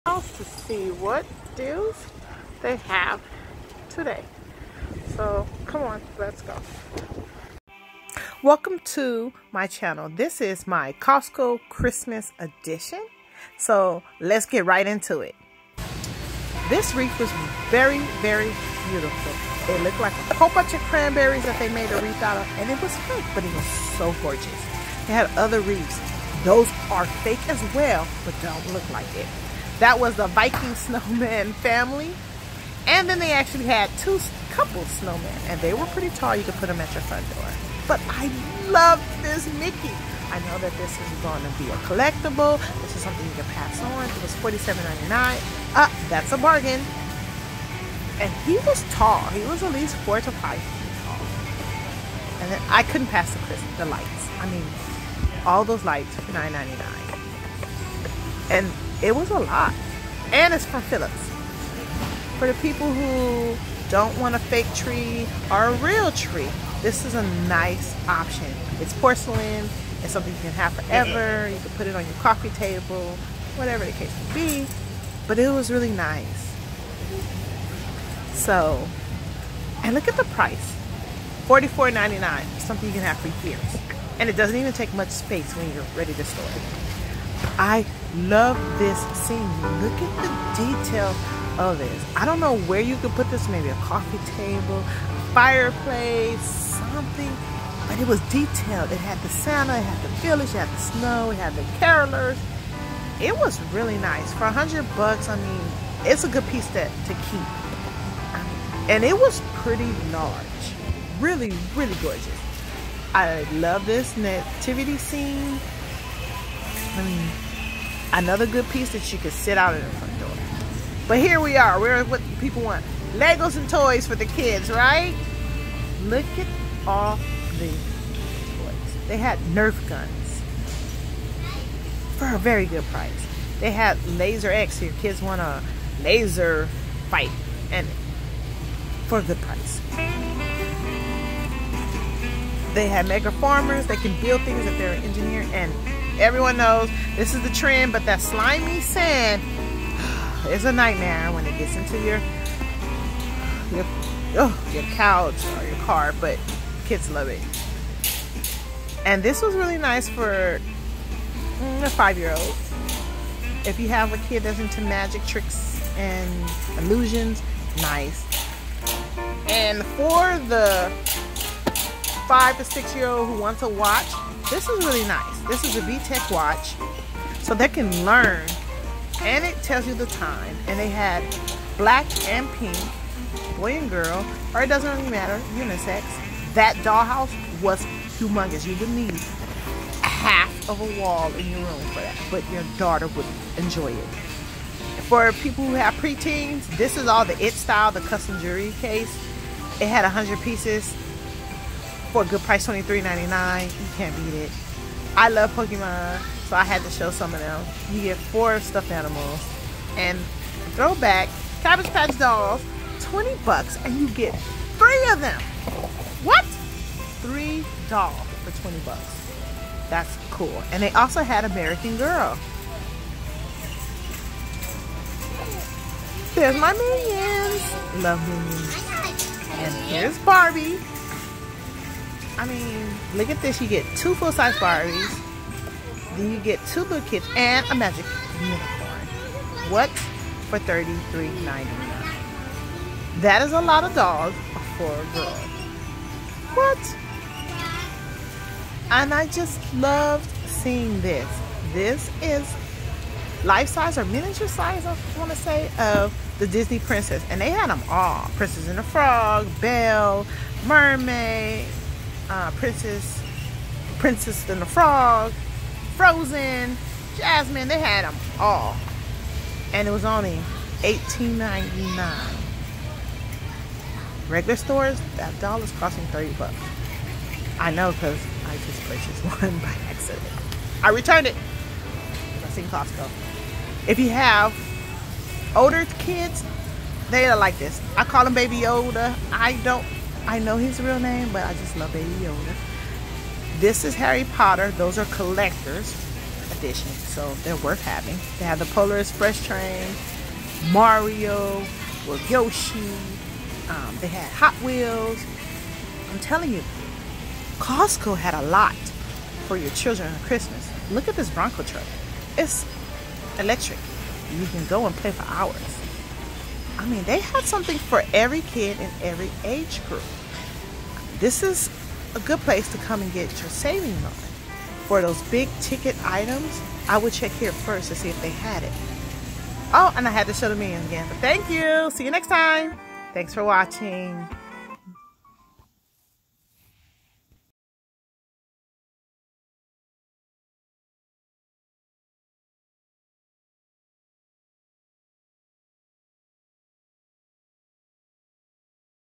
The INSANE Christmas finds at Costco in 2022!
Costco is really bringing the Christmas spirit to there store with all these great finds.